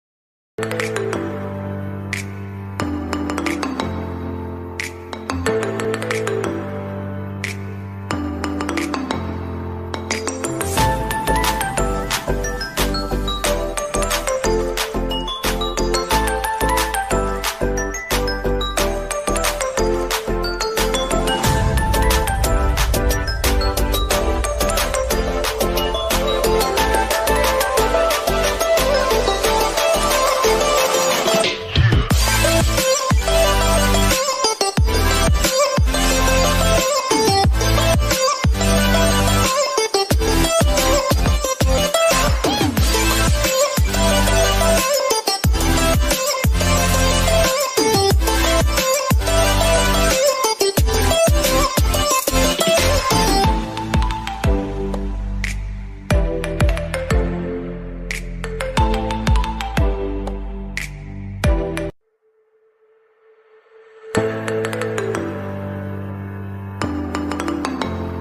Tiranga Bike Headlight making at home easy
15 August special

Make amazing India National flag head light at home
easy